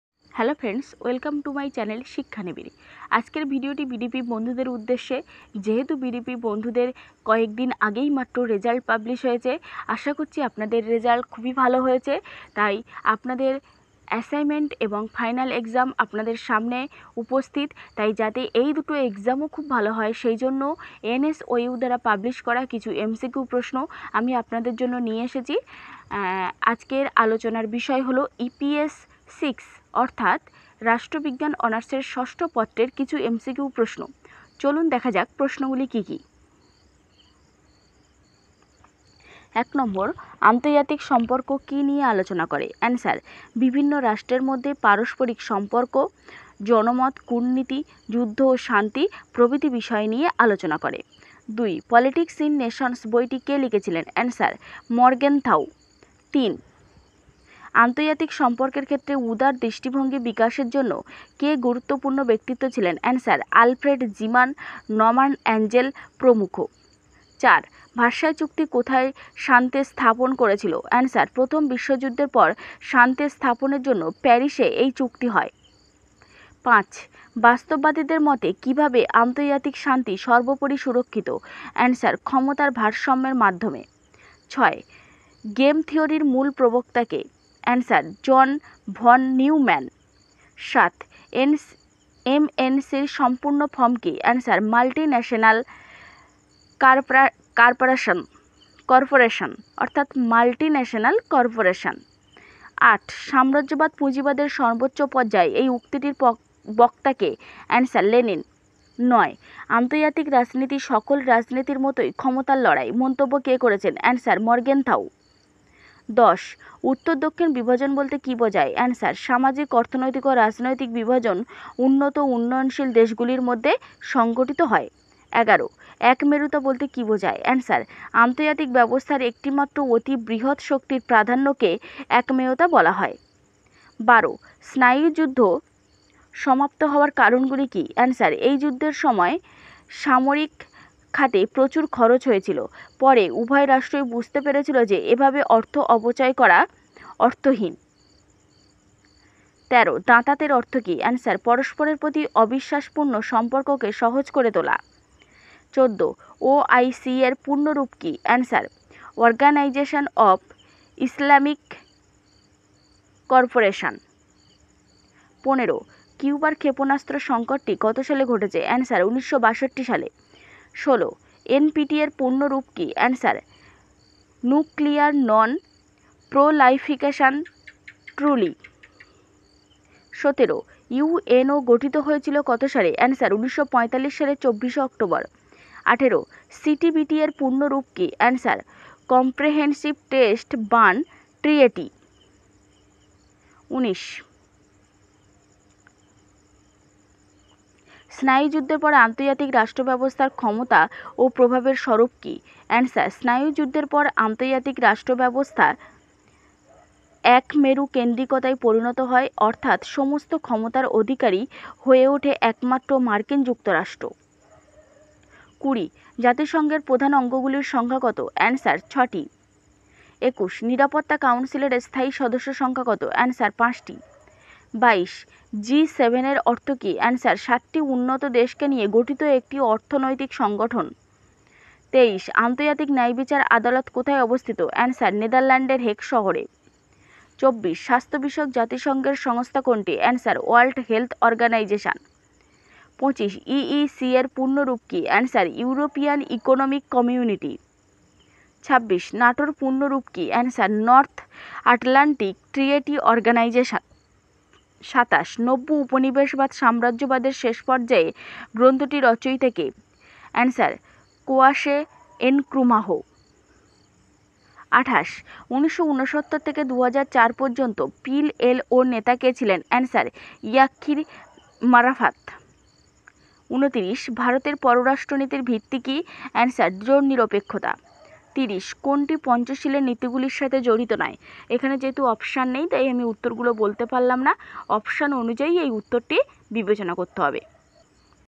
Political Science Assignment Practice Set 2021 । EPS - 6 Final Exam Suggestion 2021 ।
📌 Video তে যদি 100 লাইক আসে, তাহলে পিডিএফ লিঙ্ক Description এ দিয়ে দেবো ।

Our Motive is to provide you The Best Study Material, Notes and all Subject Suggestion 'Totally Free'.